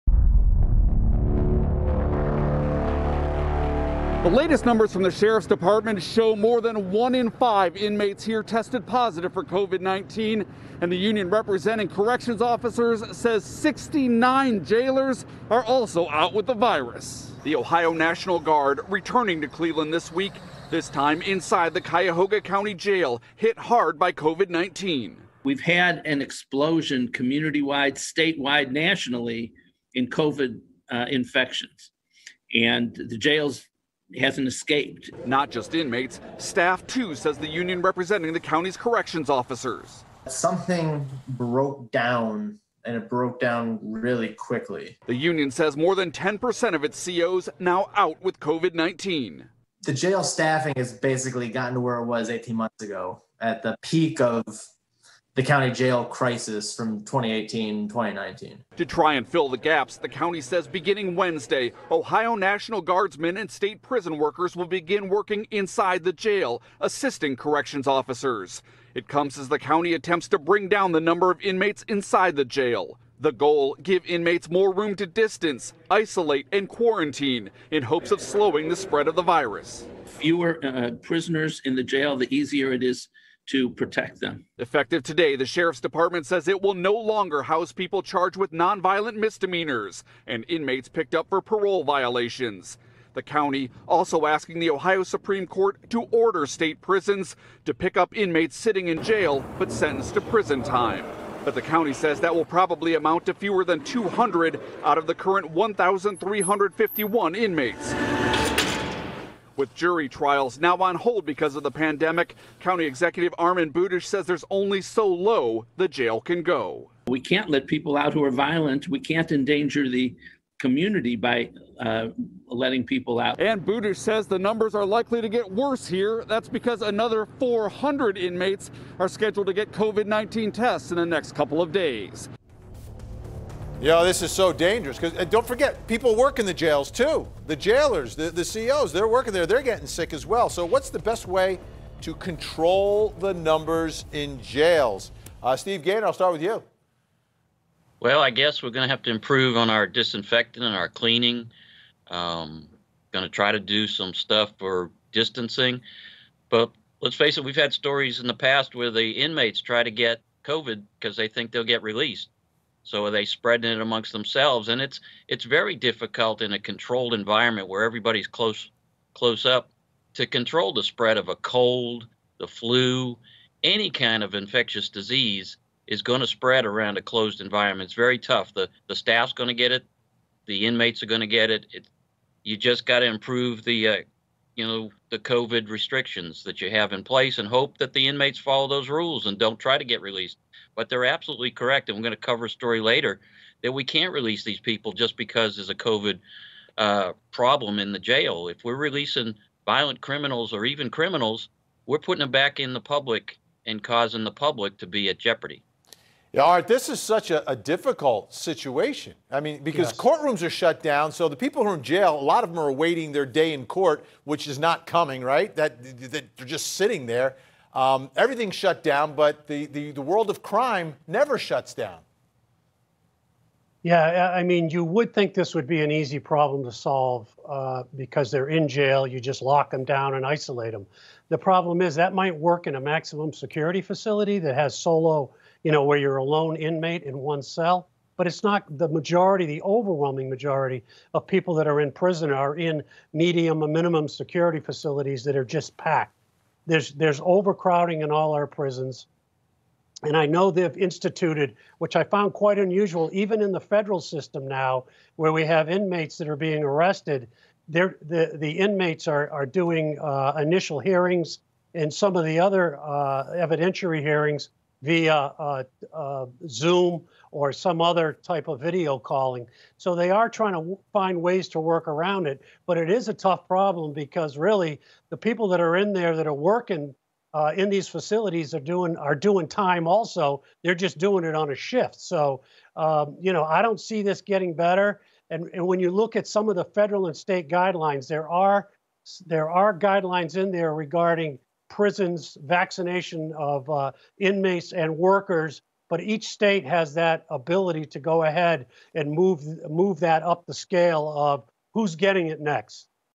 COVID Crisis Climbing in Prisons | 1-in-5 Inmates Test Positive | COURT TV
COVID IN JAIL: According to reports, 1 in every 5 state and federal prisoners have tested positive with the coronavirus.

What do YOU think is the best way to control the numbers in jail?